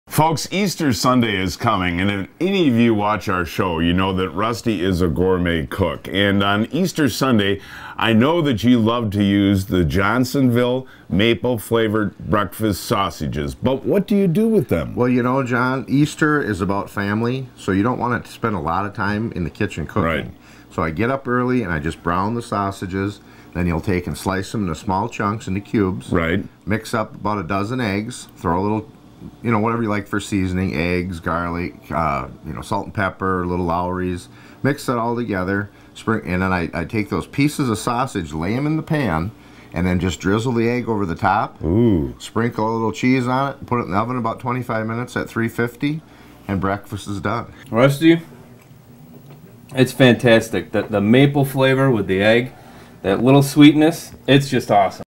Johnsonville Breakfast Egg Bake
Rusty Lilyquist shows us his Johnsonville Breakfast Egg Bake, a great breakfast for easter.